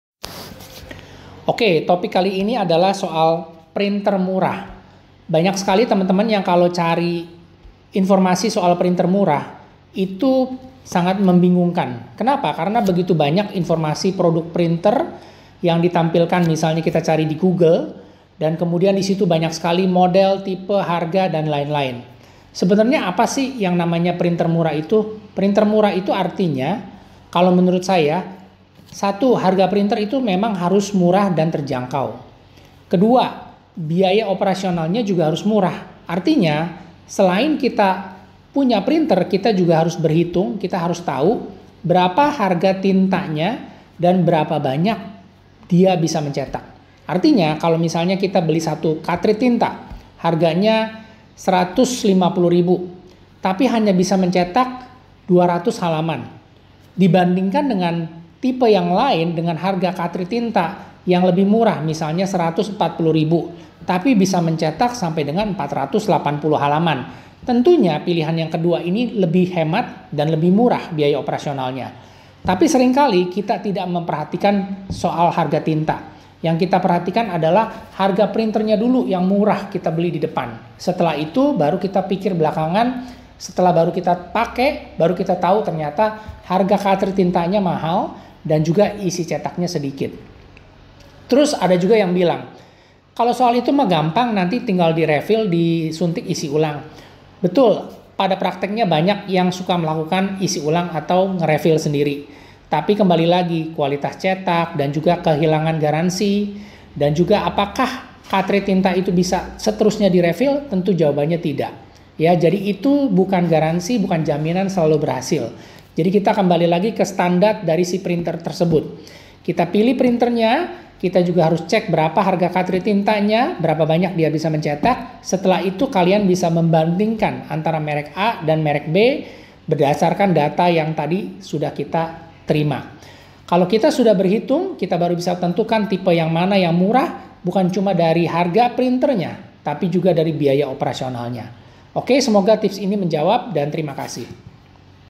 Printer Murah itu seperti apa? Hal Apa yg perlu di perhatikan pada saat ingin membeli Printer?
Printer murah adalah pilihan cerdas bagi mereka yang membutuhkan perangkat cetak tanpa menguras anggaran.

Dengan harga yang terjangkau, printer murah memungkinkan pengguna untuk mencetak dokumen, foto, atau materi lainnya dengan kualitas yang memadai. Meskipun tidak dilengkapi dengan fitur canggih, printer ini tetap bisa menjadi solusi yang efektif terutama untuk penggunaan di rumah atau kantor kecil.

Keuntungan lainnya adalah biaya perawatan dan perbaikan yang rendah, menjadikannya pilihan yang ekonomis dalam jangka panjang. Namun, pengguna tetap perlu memperhatikan kualitas hasil cetak untuk memastikan kepuasan penggunaan jangka panjang.

Dengan demikian, printer murah dapat memberikan solusi cetak yang praktis dan hemat tanpa mengorbankan kualitas.

** Kalau ada pertanyaan yg belum di jawab, Bisa comment atau tanya langsung di Group